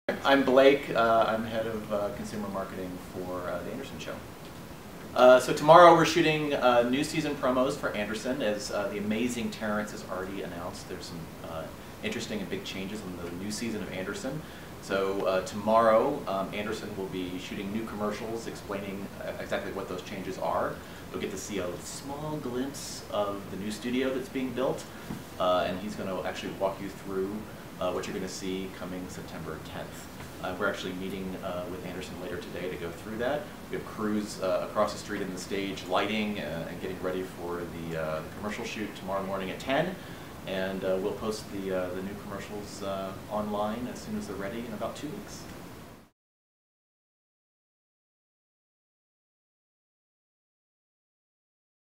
Blake of the 'Anderson' Marketing Team Talks Promo Shoot
The "Anderson" team is busy preparing for a Season 2 promo shoot with Anderson... and Blake, in charge of consumer marketing, gives the inside scoop on what will happen at the shoot.

Be sure to tune in for the season premiere on Sept. 10.